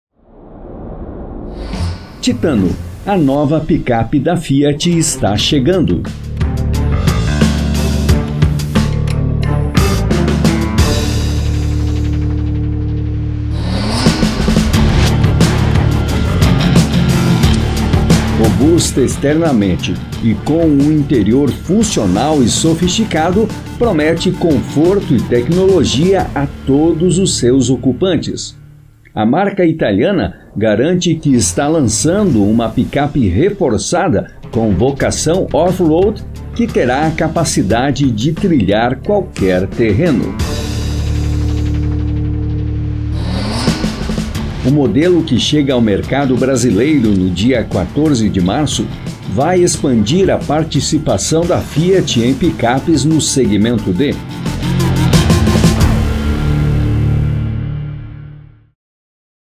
FIAT TITANO 2024
Fiat lança no próximo dia 14/03 a Titano, sua nova picape do segmento D.